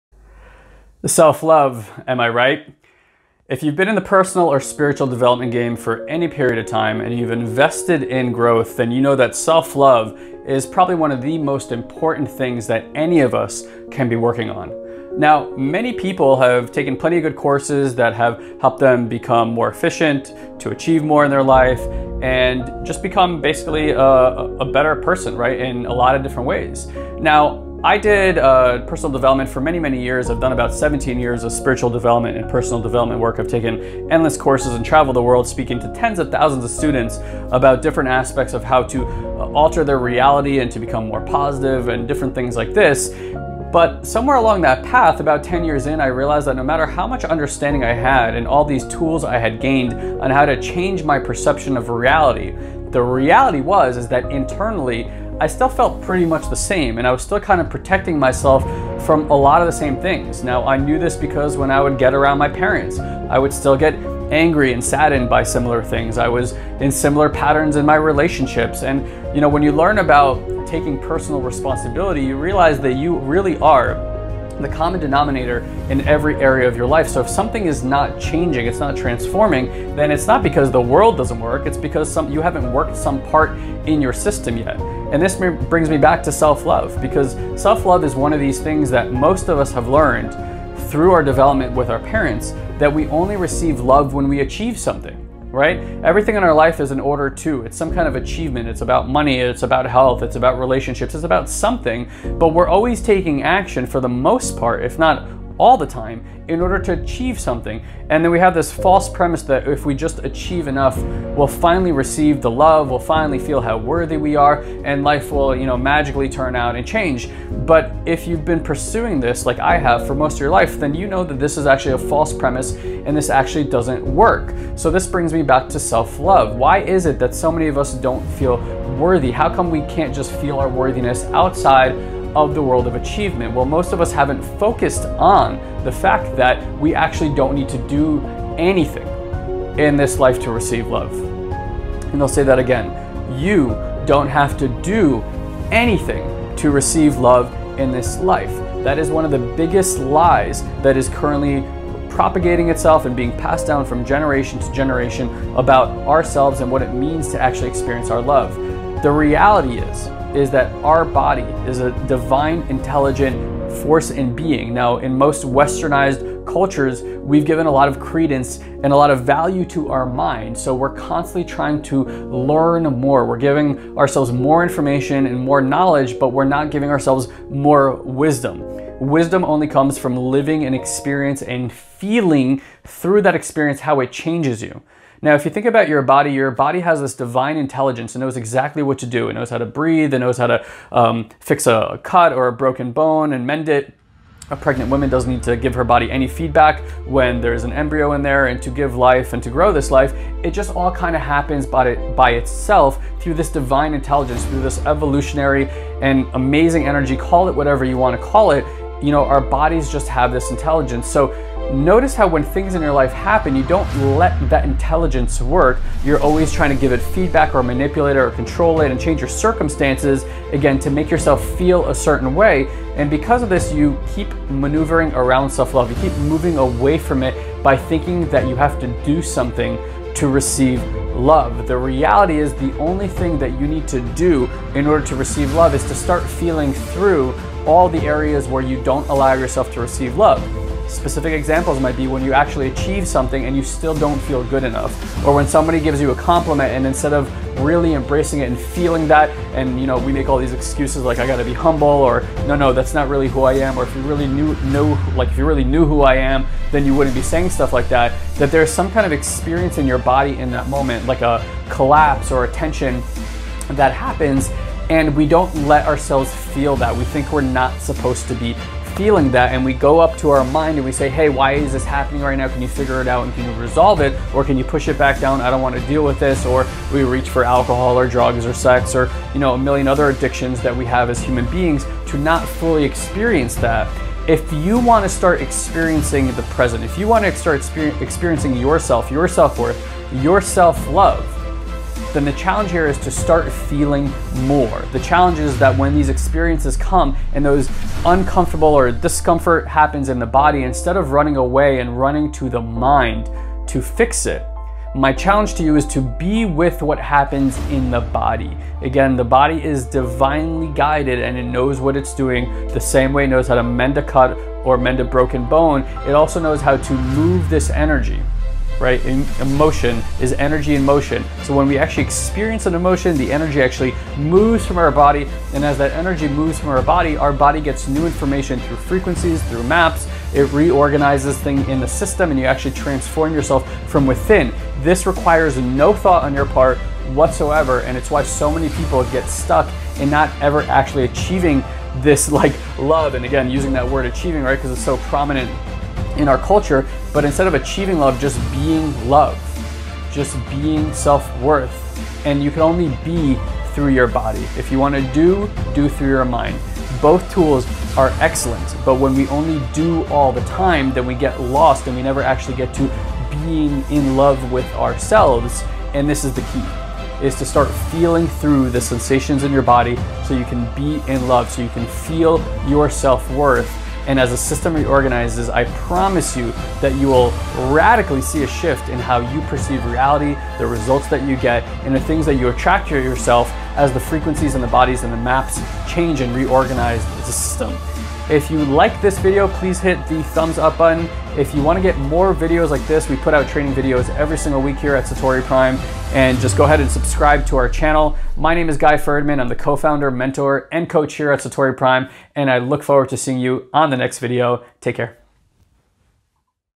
How To Love Yourself More - Self-love - Personal Development
Listen to why loving yourself more is drastically underrated. Guy Ferdman shares that self-love is NOT something that most of us have learned through our development with our parents, rather we've learned that we only receive love when we achieve something.

➤➤➤ START YOUR TRANSFORMATION NOW is a unique group of people that crave knowledge, seek unconventional wisdom, that are fed up with a mundane way of living.